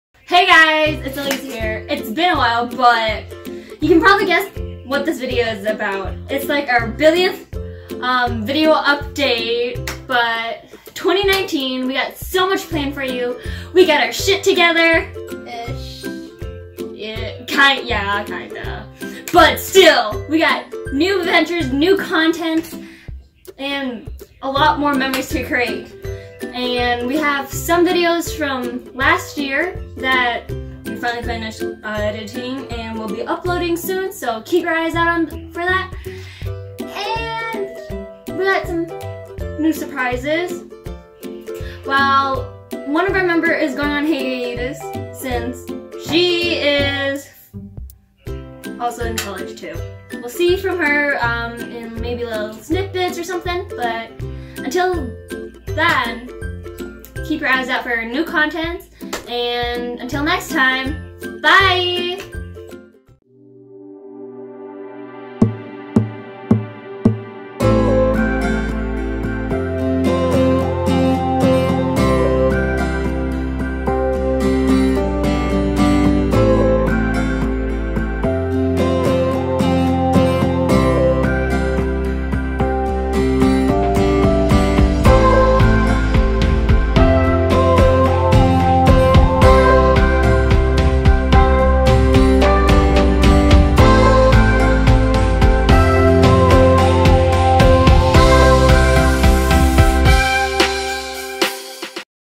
lol another update video cuz 2019 and we got our sh*t together this year
happy 2019!
we actually have stuff planned out for this year and we aren't gonna post a video and then go missing for another year again LOL. we have exciting news! a new member joined the MPB team and we are excited to introduce her! stay tuned for that!

xoxo MPB